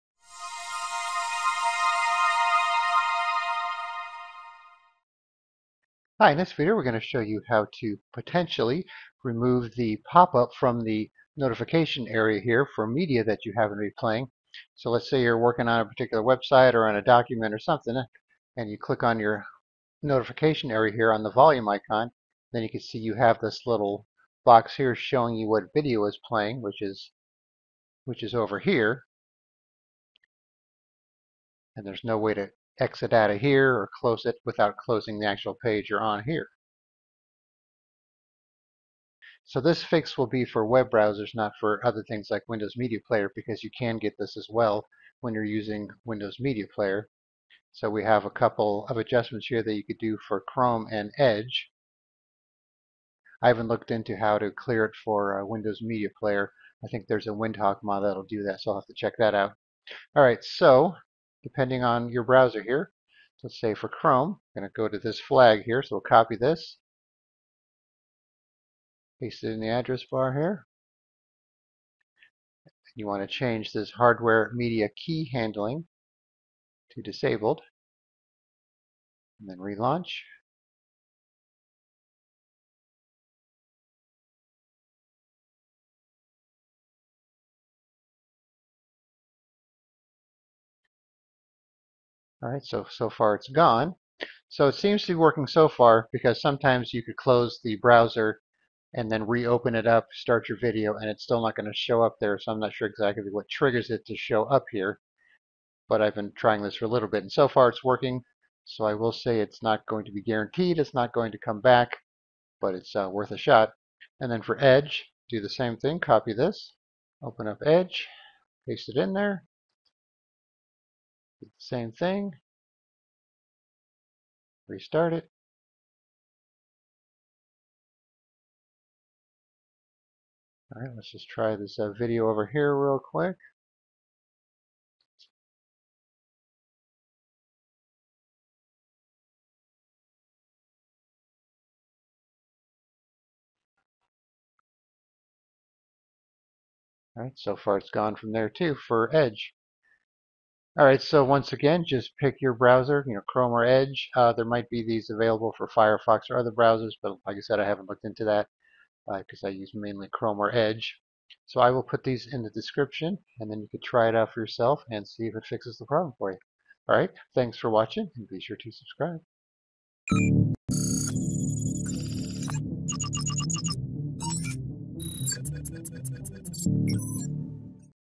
How to Disable the Media Control Overlay from the Notification Area for Chrome & Edge
**Note - If this method doesn't work then this will definitely work.

You may have noticed that when you are playing something like a YouTube video or streaming some type of music or other audio that you get a popup in your Windows notification area showing you the media that happens to be playing in your browser or other app. In this video, we will show you how to disable this flyout when playing media in Google Chrome or Microsoft Edge.

Here are the flags for Chrome and Edge.

chrome://flags/hardware-media-key-handling
edge://flags/hardware-media-key-handling